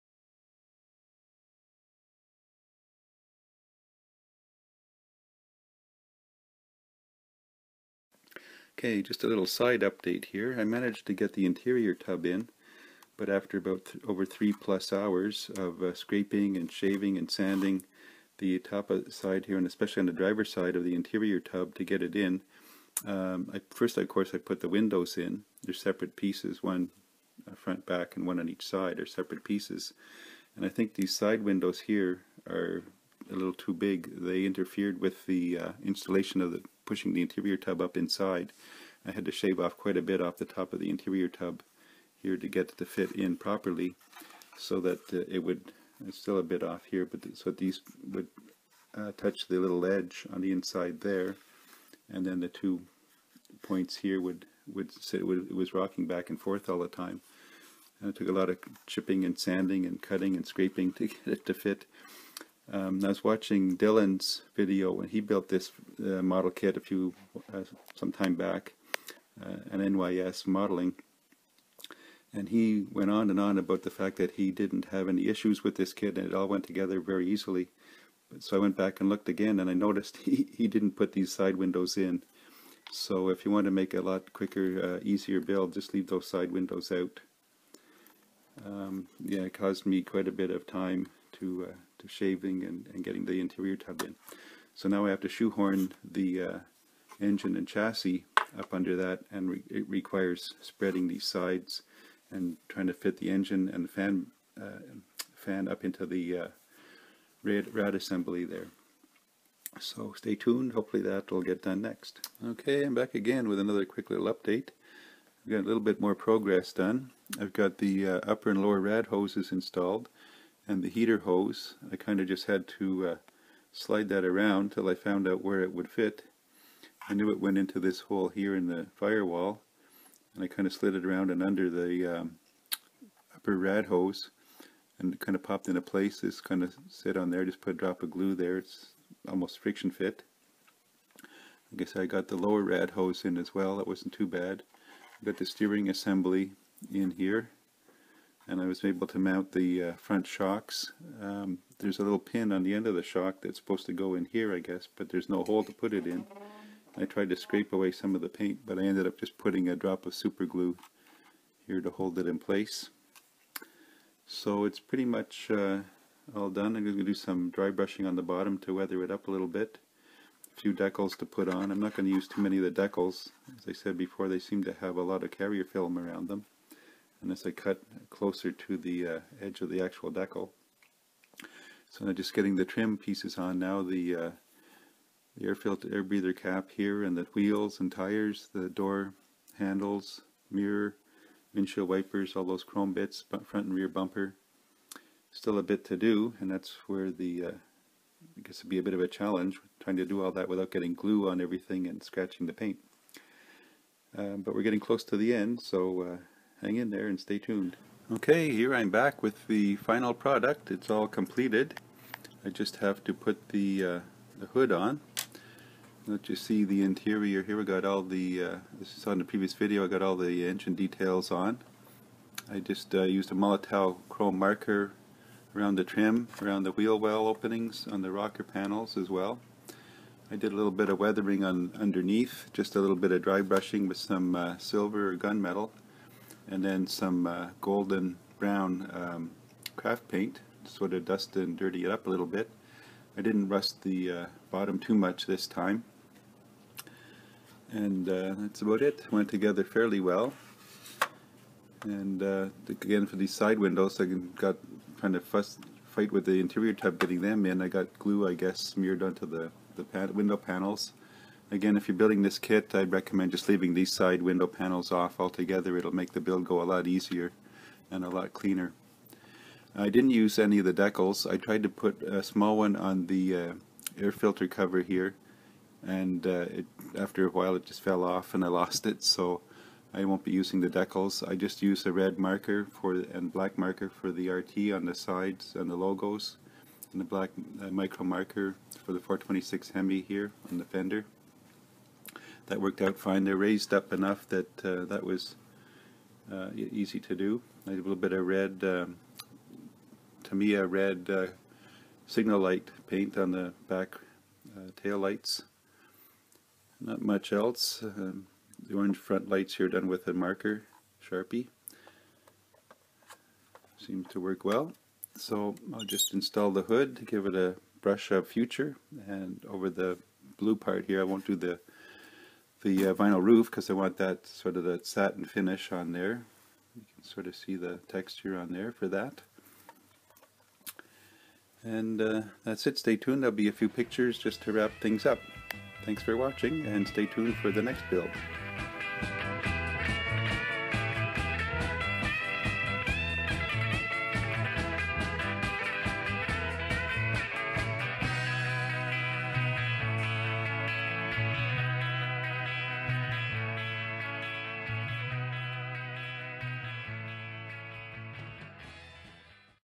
Psychedelic 60's Group Build - Revell '67 Coronet R/T 3rd Update (FINAL)
After some trouble getting the interior tub in (due to interference from the side windows) I present the completed build of the '67 Dodge Coronet R/T.